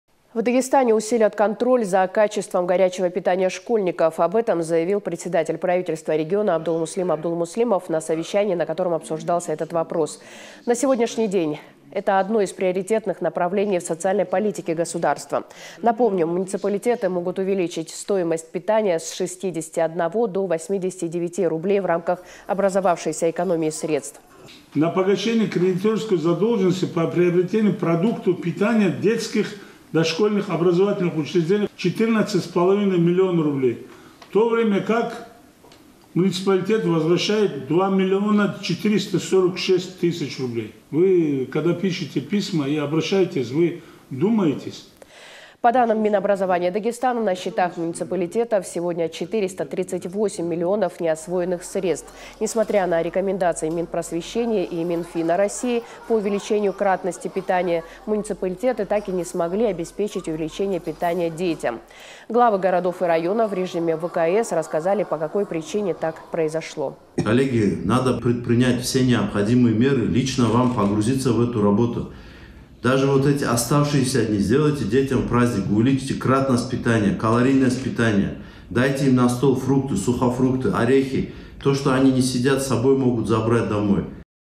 В Дагестане усилят контроль за качеством горячего питания школьников
Об этом заявил председатель Правительства региона Абдулмуслим Абдулмуслимов на совещании, на котором обсуждался этот вопрос. На сегодняшний день это одно из приоритетных направлений в социальной политике государства.